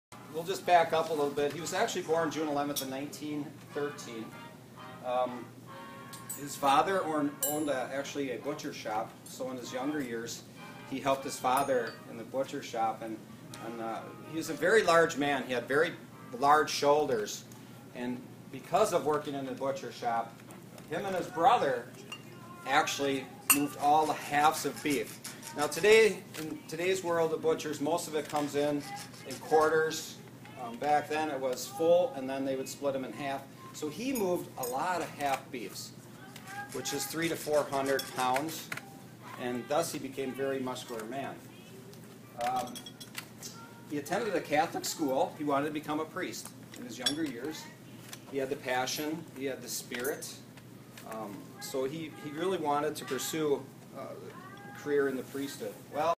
Duchow, who works at the Radisson where Vince Lombardi's is located, talks about the quality of serv
Duchow, who works at the Radisson where Vince Lombardi's is located, talks about the quality of service he expects.

By: Theresa Audia
Published on: April 29, 2014